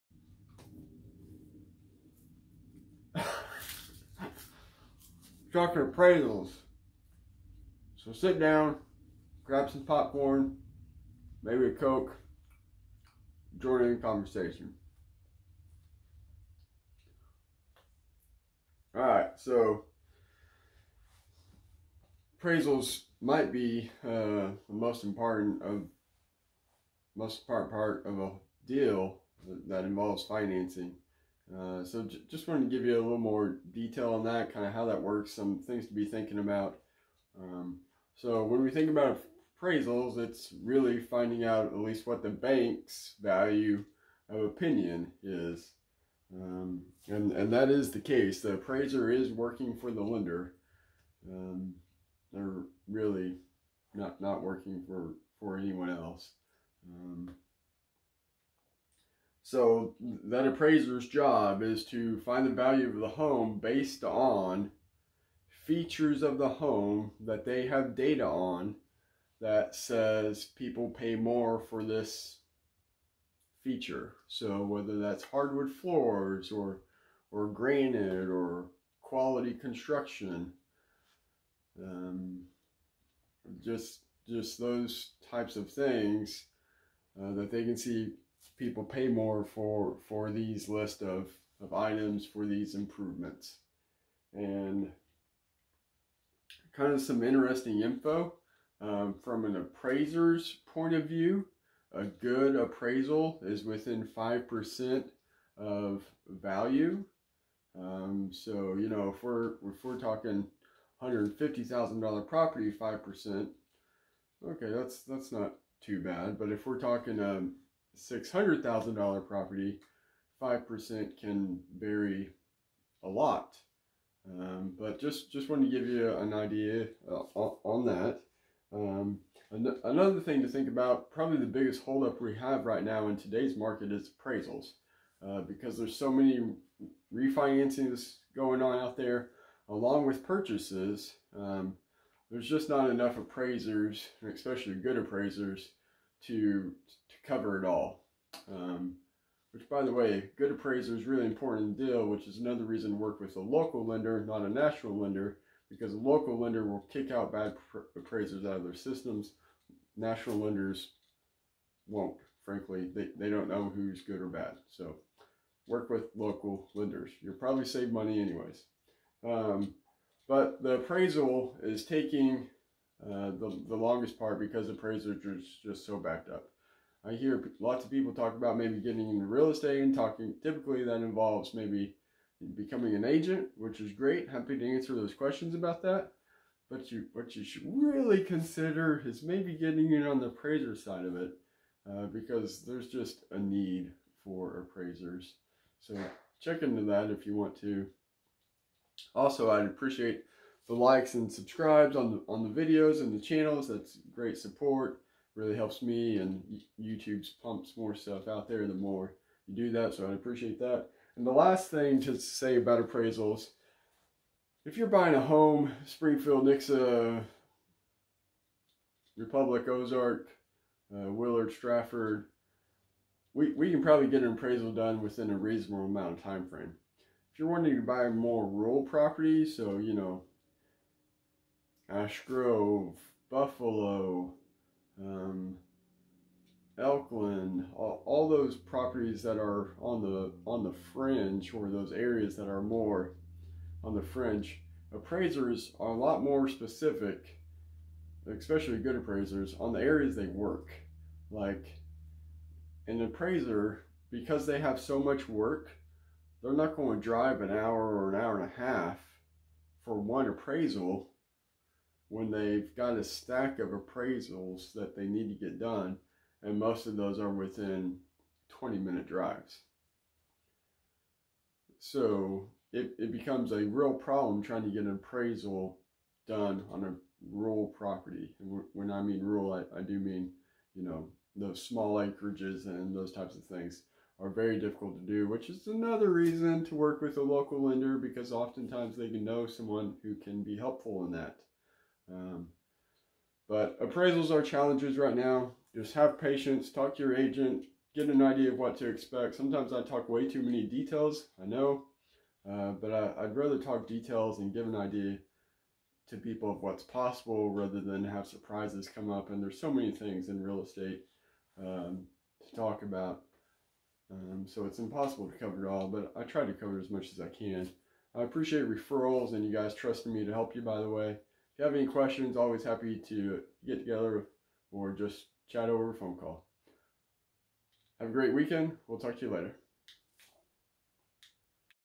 appraisals, the good & the bad
How do appraisers determine value?
If you are purchasing in areas like Ash Grove or Buffalo how that could impact the process vs purchasing in Springfield or Republic.